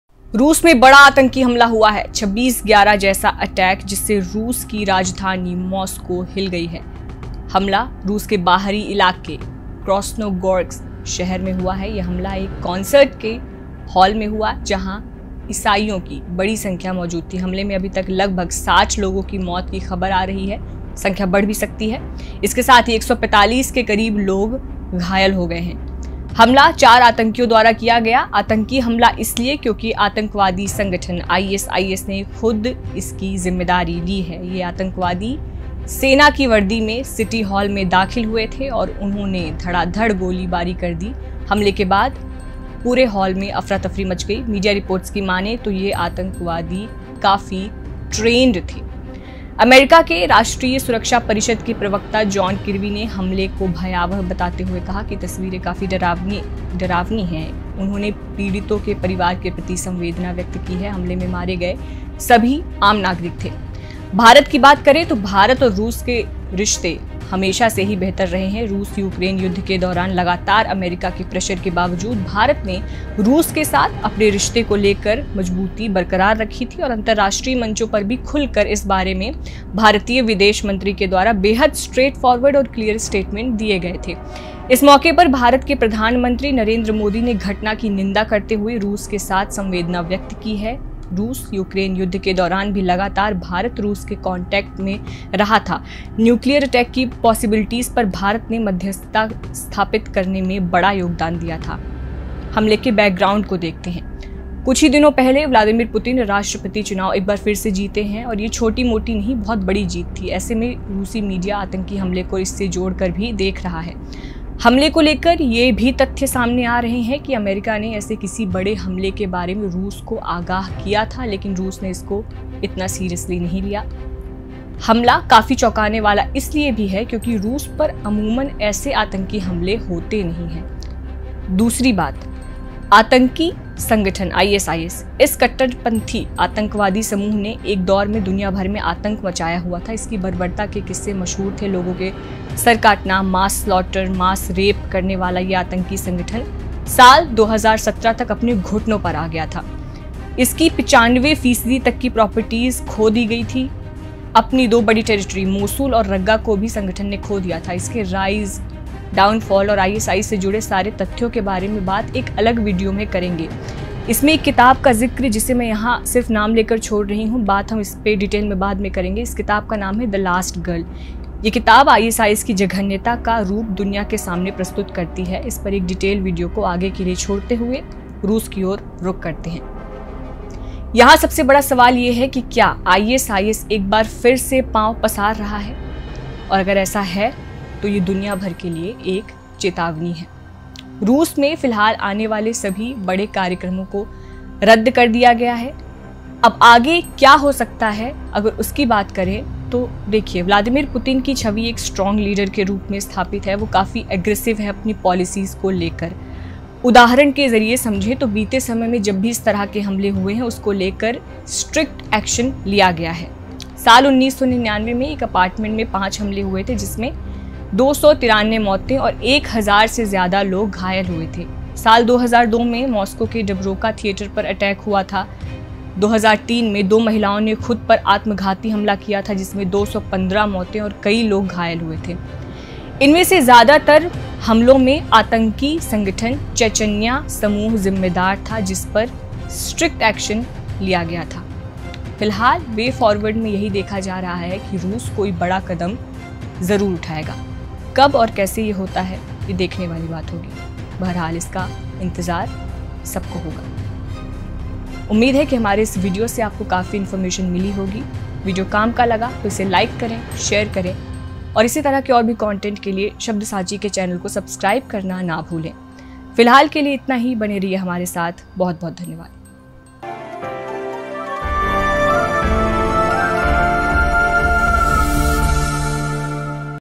Moscow Concert Hall Attack | ISIS Claims Responsiblity | Vladimir Putin | India Reaction
रुस में बड़ा आतंकी हमला हुआ है जिससे रुस की राजधानी मॉस्को हिल गयी है.हमला रूस के बाहरी इलाके क्रास्नोगोर्स्क शहर में हुआ है.ये हमला एक कॉन्सर्ट हाल में हुआ है जहाँ इसाईओं की बड़ी संख्या मौजूद थी.हमले में अभी तक लगभग 60 लोगों की मौत की खबर आ रही है.इसके साथ ही 145 के करीब लोग घायल हो गए हैं.हमला 4 आतंकियों द्वारा किया गया है.आतंकी हमला इसलिए क्योंकि आतंकवादी संगठन ISIS ने खुद इसकी जिम्मेदारी ली है.ये आतंकवादी सेना की वर्दी में सिटी हॉल में दाखिल हुए थे और इन्होने धड़ाधड़ गोलीबारी कर दी. हमले के बाद पूरे हाल में अफरा तफरी मच गयी .मीडिया रिपोर्ट्स की मानें तो ये आतंकवादी काफी ट्रेंड थे.